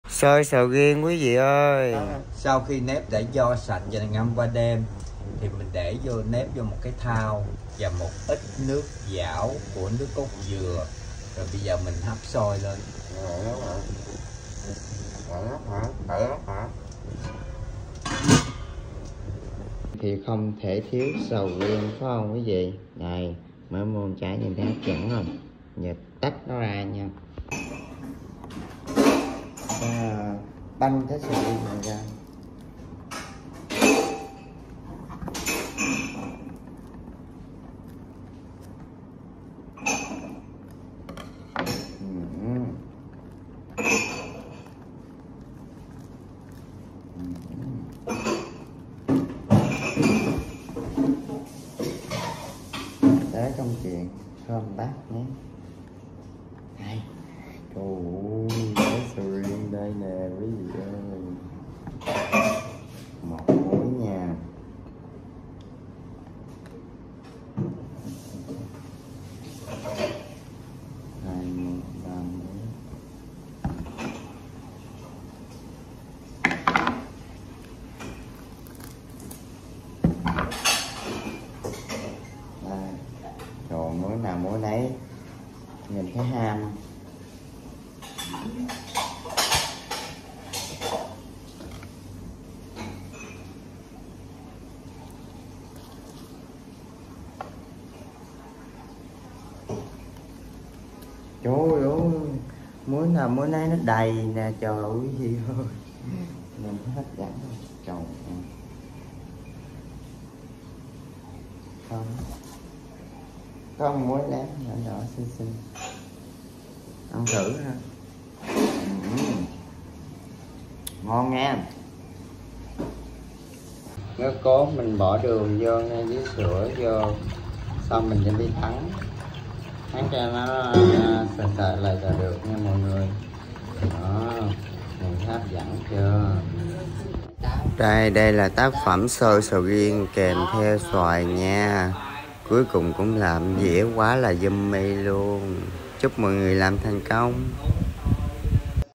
Món ăn ngon - Xôi sầu riêng nhà làm
Hôm nay nhà làm xôi sầu riêng kiểu thái lan , lần đầu làm mà được như vậy quá là ngon và tuyệt vời
Cảm ơn tất cả mọi người đã xem ủng hộ kênh
Chúc cả nhà ngày mới vui vẻ